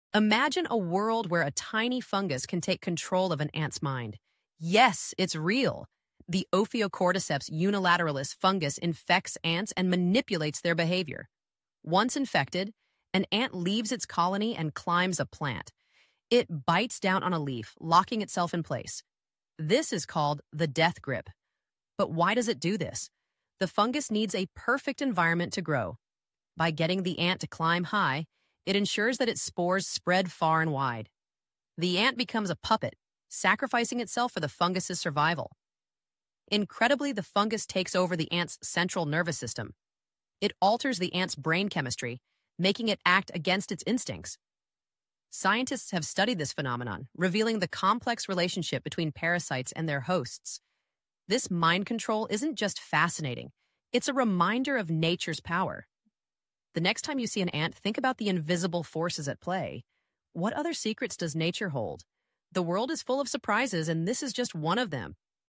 Zombie Ants: The Fungus That Takes Control
A fungus that mind-controls ants?
Meet Cordyceps — a parasite that invades an ant’s body, hijacks its brain, and forces it to climb and die… just so it can spread.
Nature has horror stories — and they’re all real.
New curiosity every day! 🧠🐜